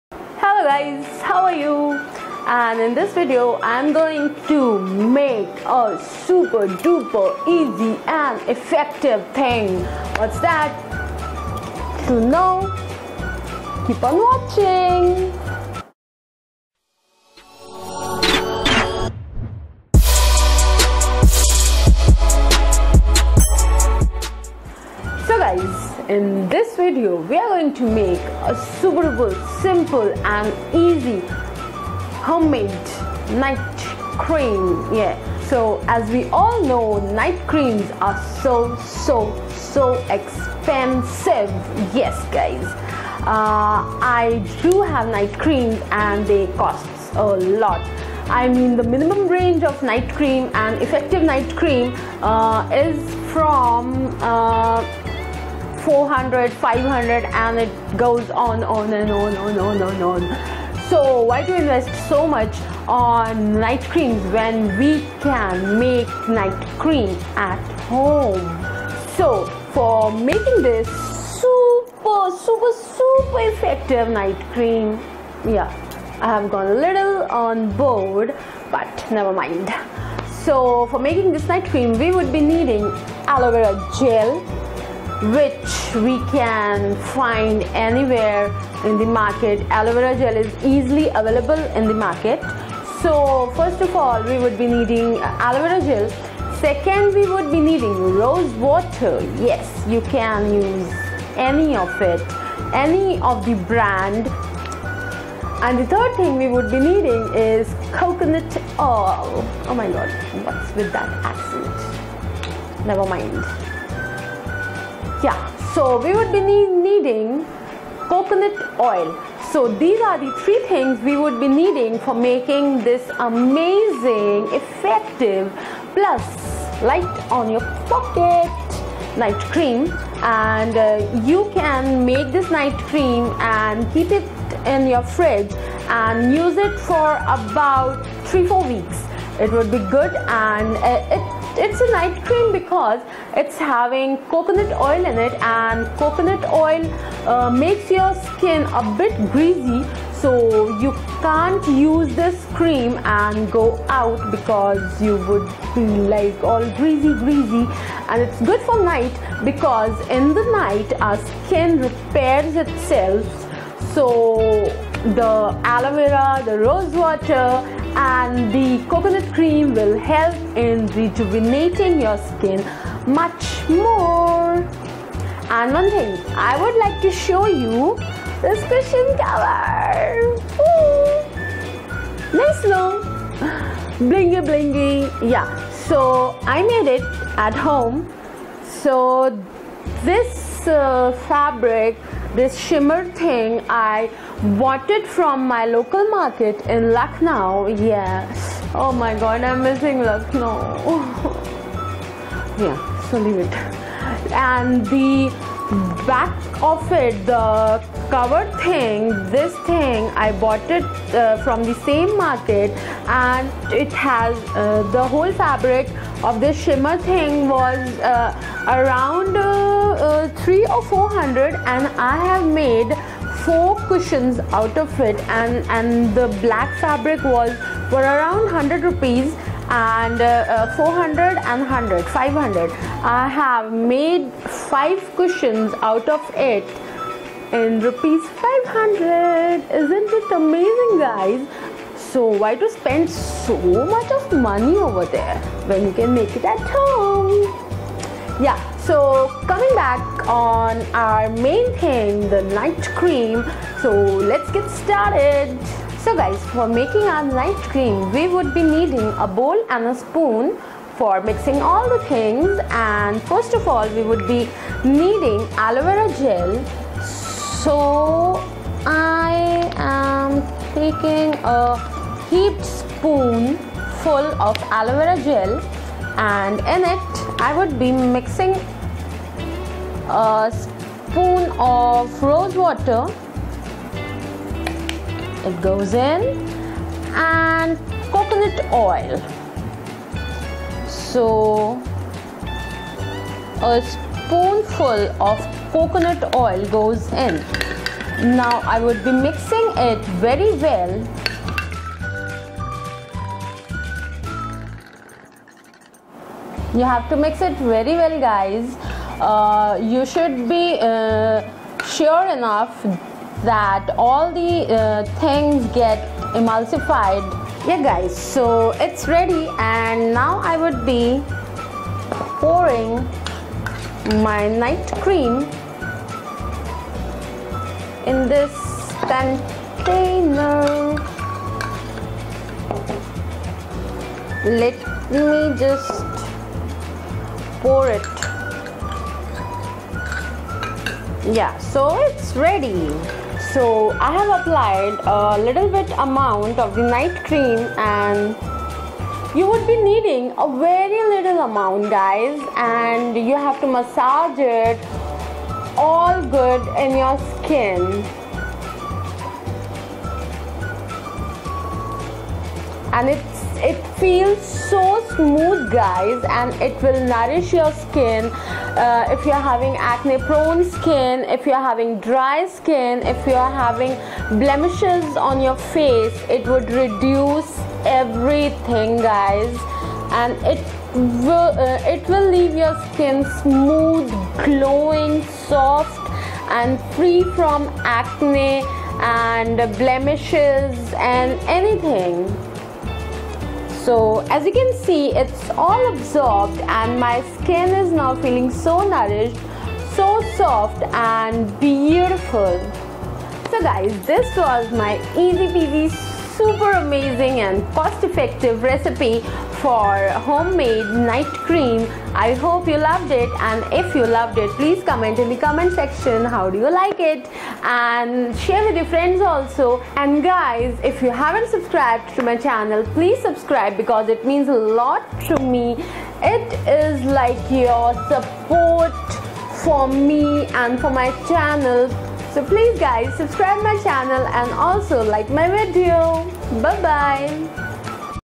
Homemade Night Cream | How To Make Night Cream For Glowing And Younger Skin At Home | Indian Makeup
Hi Babes, in this video, I will be showing the best DIY NIGHT CREAM which you can make at the comfort of home and is completely natural. This night cream is best for all skin types, and with the regular usage, you will get visible results in reduced acne marks, blemishes, and pigmented skin.

Stay blessed
Stay happy 